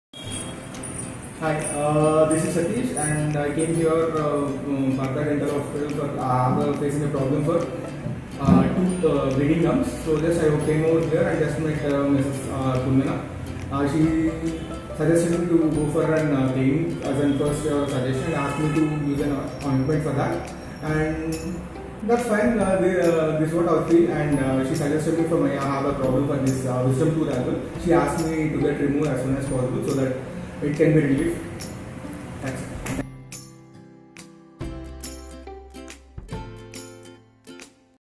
Happy patient TESTIMONIALS AFTER treatment of Bleeding Gums,II Partha Dental II
hen individuals don't hone appropriate dental cleanliness, microscopic organisms in the mouth shapes plaque on the teeth. These microbes may make your gums wind up aggravated, which brings about red, swollen, or draining gums. For some individuals with gingivitis, this irritation isn't difficult. ... gums that seep amid and after toothbrushin